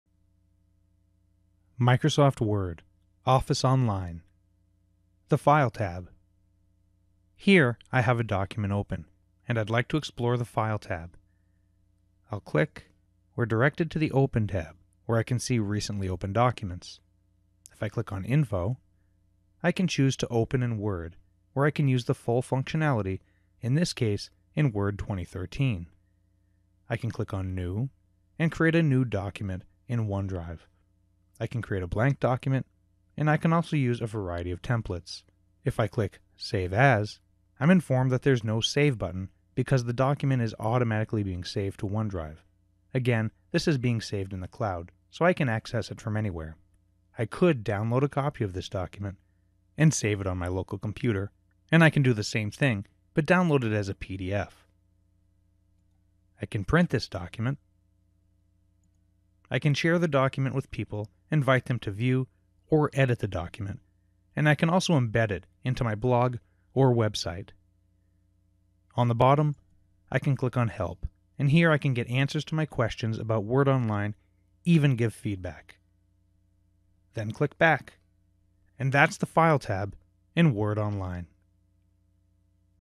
Office Online Word: The File Tab
(Duration: 01:23) Just like the desktop Office applications, Office Online has a File Tab.  This brief tour shows of the backstage view demonstrates how you can create, open, print, share, and download a copy of your document!